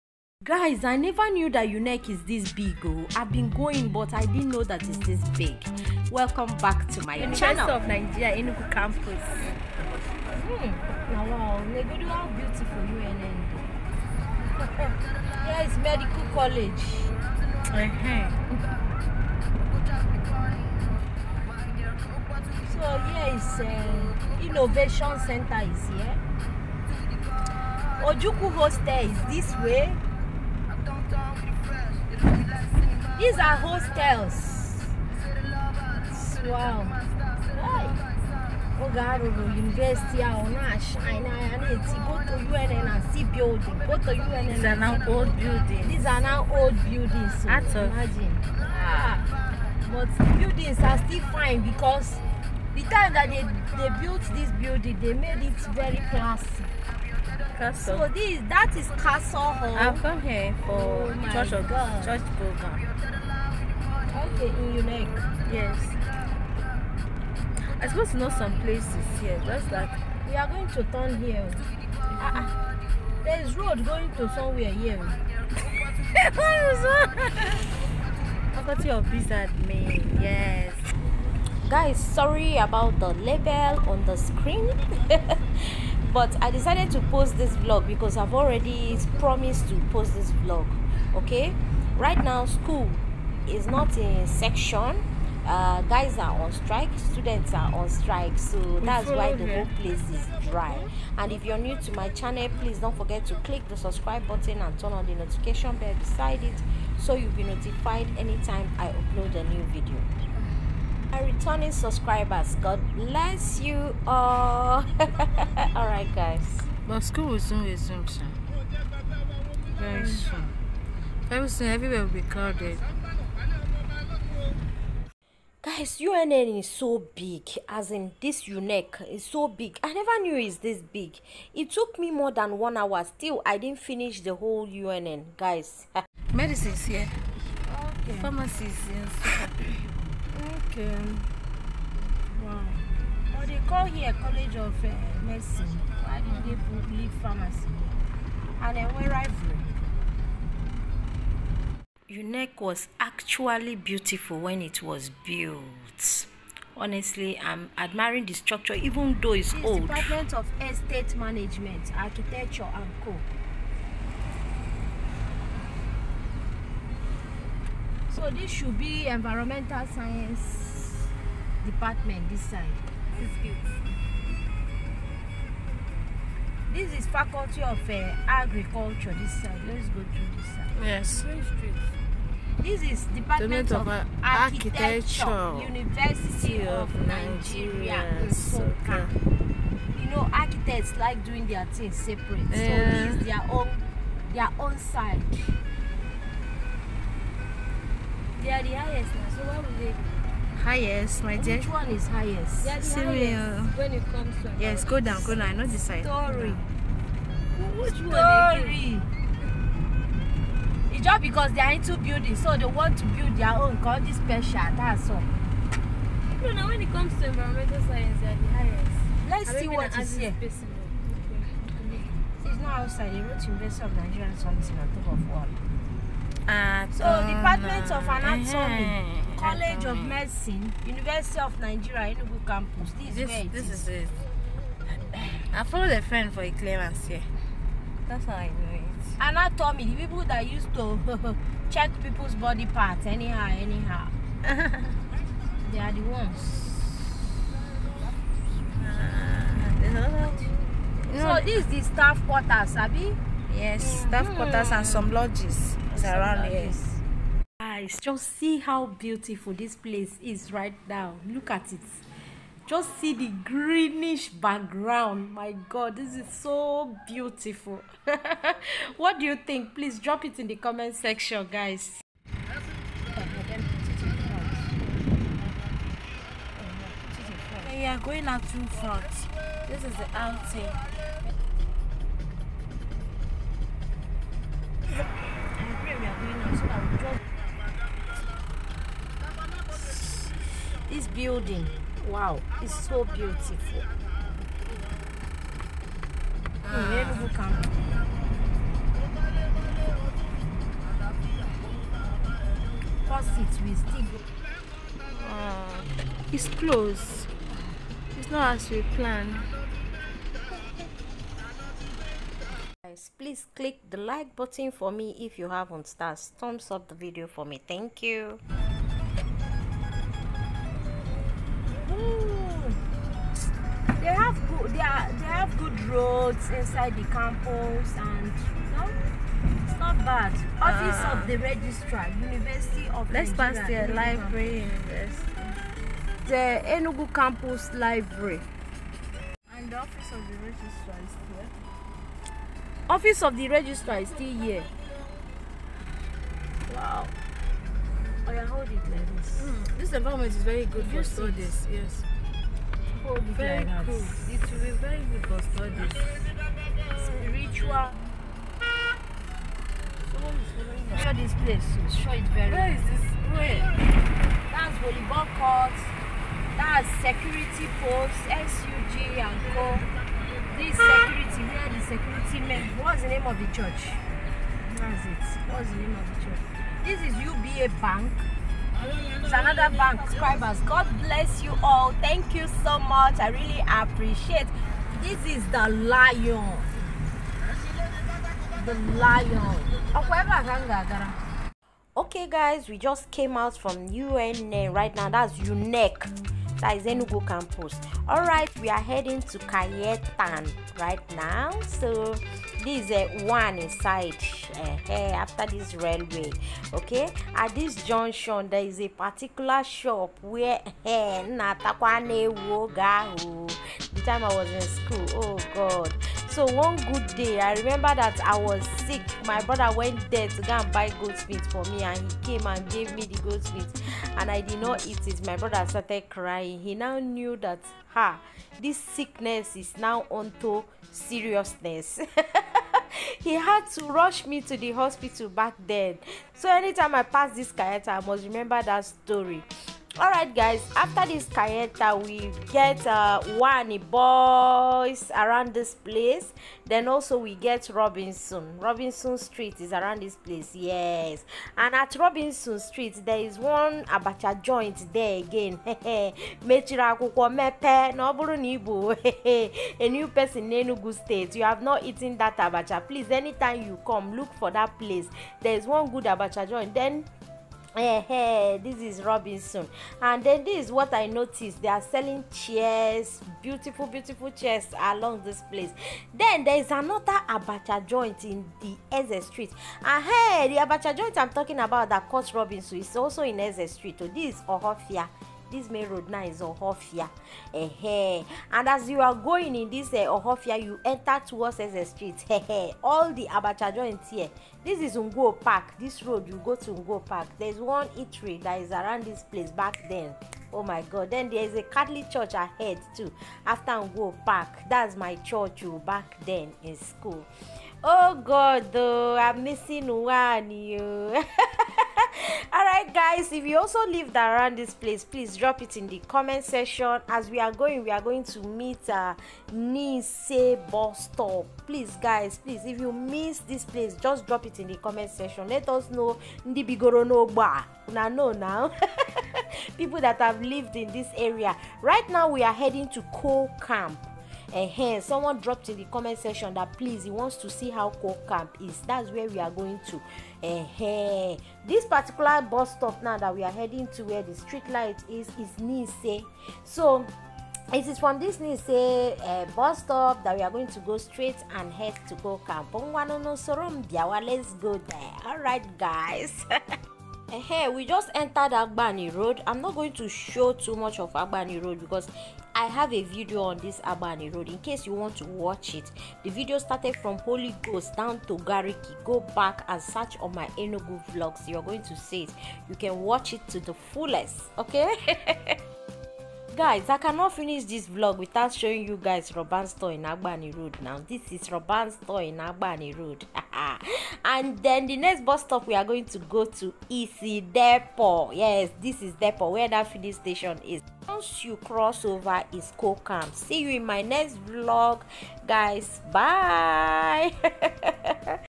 UNIVERSITY OF NIGERIA ENUGU CAMPUS, UWANI AND AGBANI ROAD
In this vlog we drove through the lonely university of Nigeria Enugu campus then finally along UWANI in Enugu state then ended at Gariki Agbani road, heading to coal camp Enugu.
I really hope you enjoy this drive by city vlog.
Please don't forget to click the like button for me, also share if you can.
God bless you more.